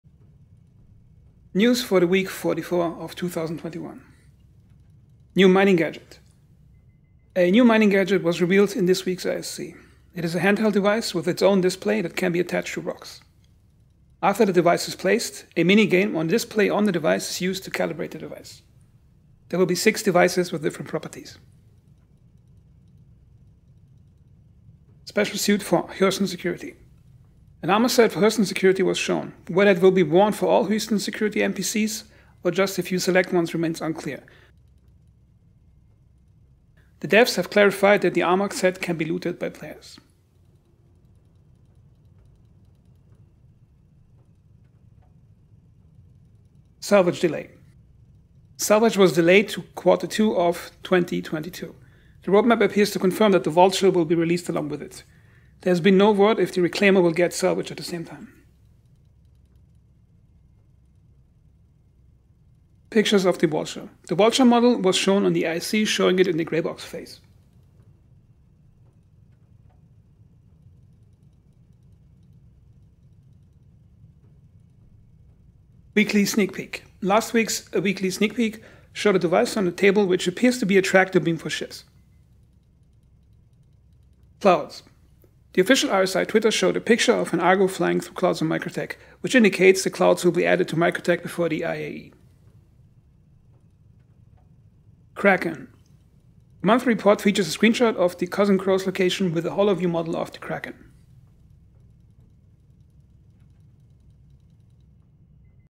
Salvage delay, mining gadget, clouds, hurston armor | Star Citizen Weekly News | Week 44 2021
This week's news:

+ new mining gadget
+ new hurston security armor set
+ salvage delay
+ clouds
+ kraken